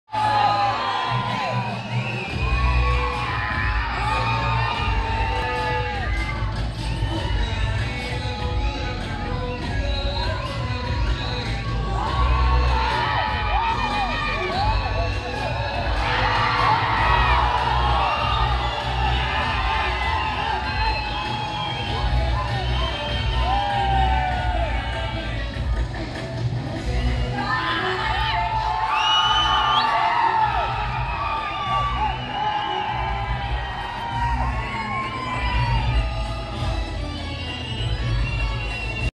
Rekka Audio Launch | Vijay Sethupathi on to the stage | Fans Mass
Vijay Sethupathi on to the stage at Rekka audio launch